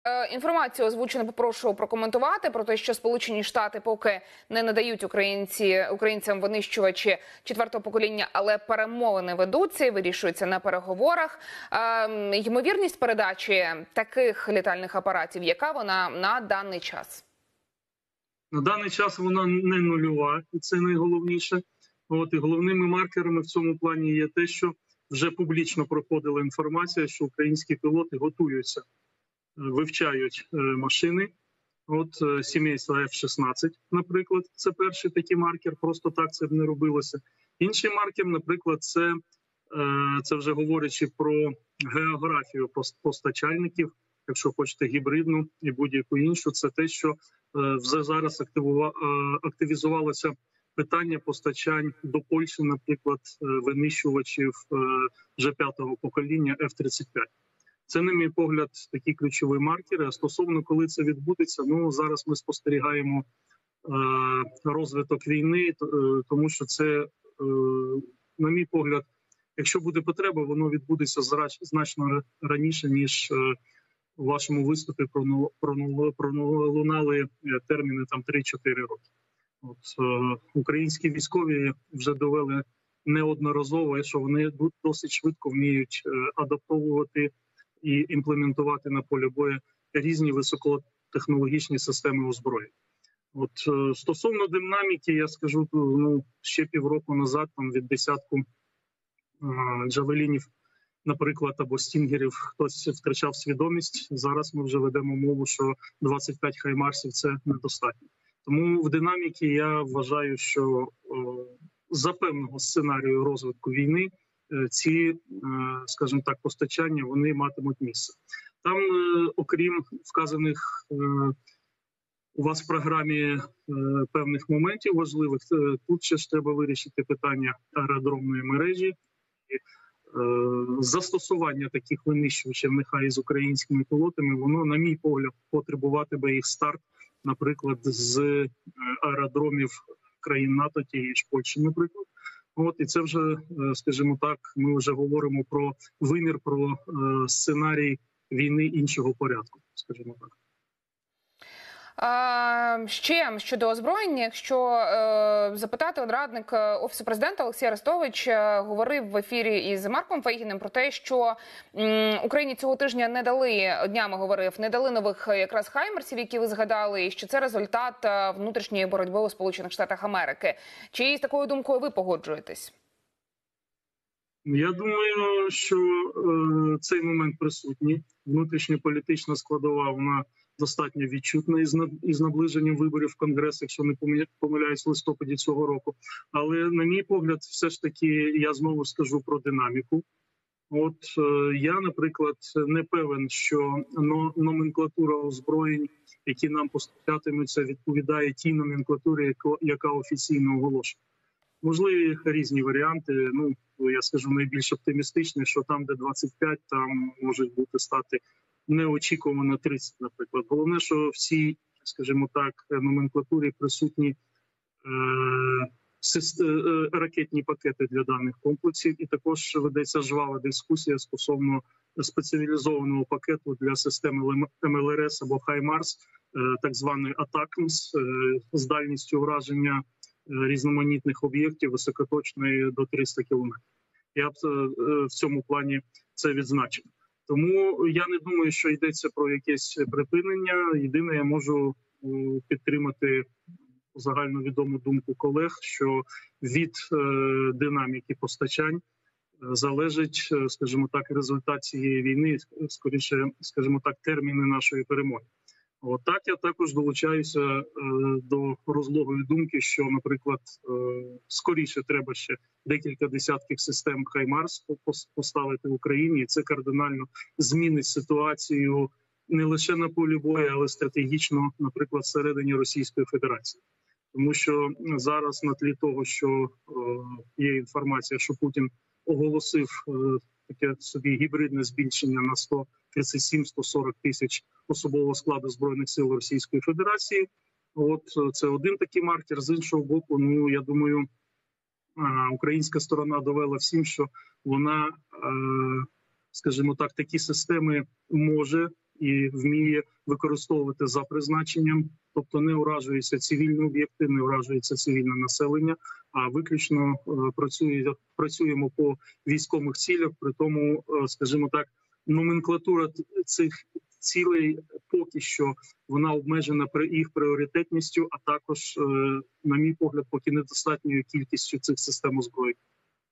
ВИНИЩУВАЧІ ДЛЯ УКРАЇНИ: українські пілоти вже вивчають американські F-16 // КОПЧАК
В ефірі: Володимир Копчак, керівник Південно-Кавказької філії Центру досліджень армії конверсії та роззброєння (Тбілісі)

24 лютого росія напала на Україну!